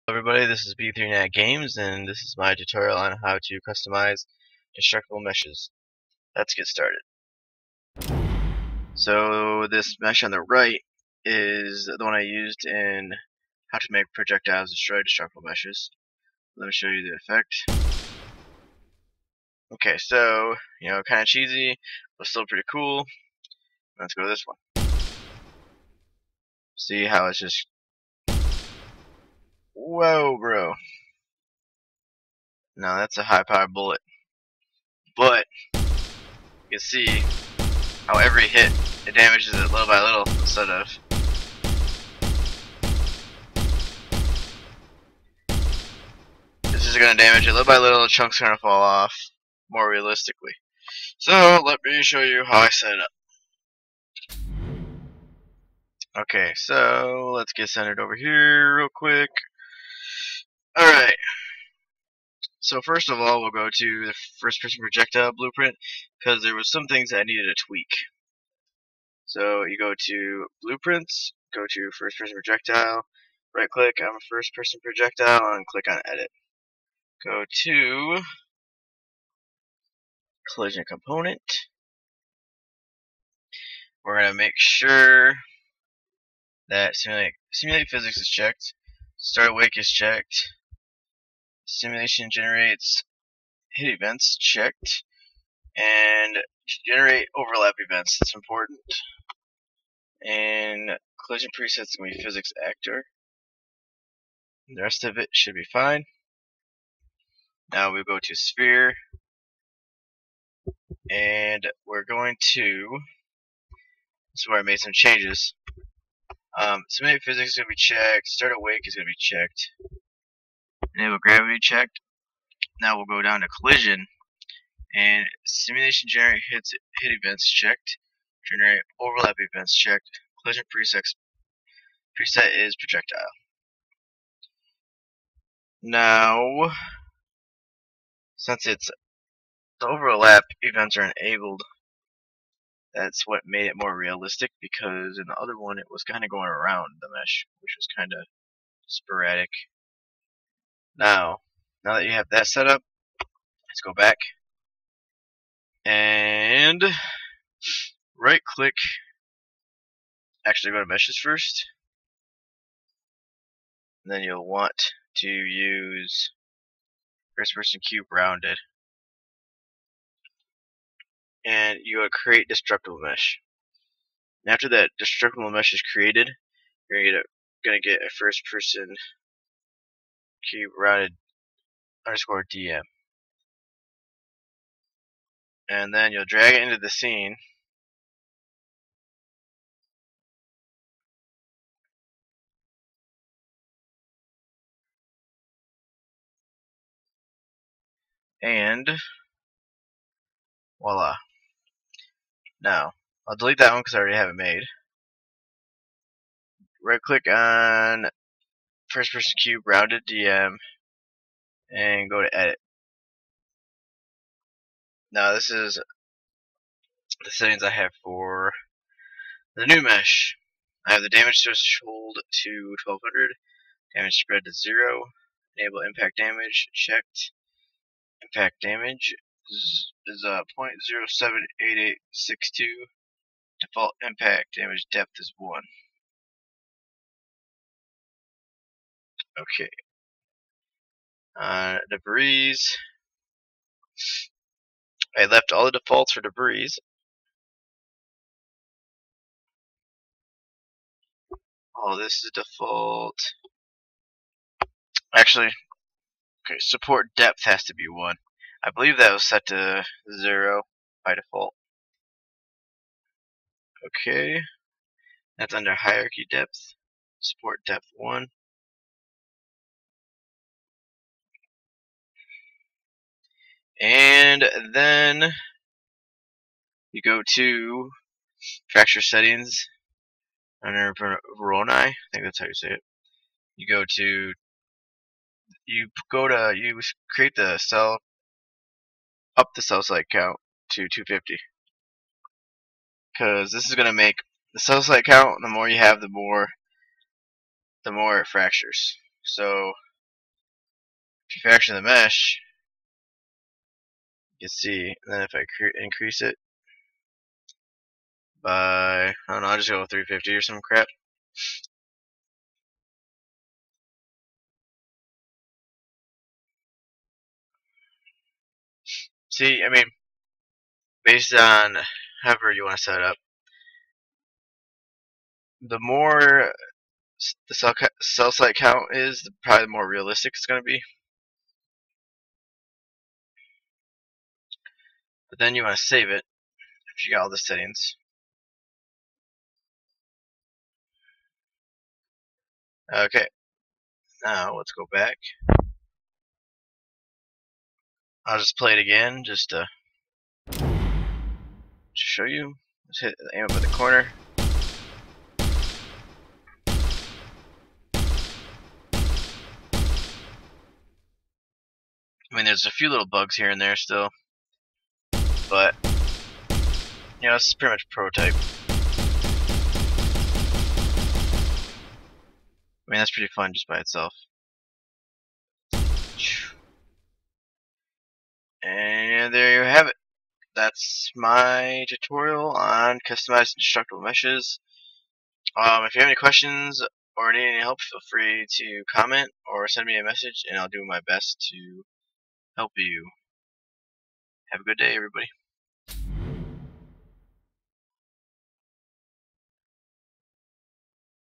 Unreal Engine 4.7.5 - Customizing Destructible Meshes
This is my tutorial on how to customize destructible meshes.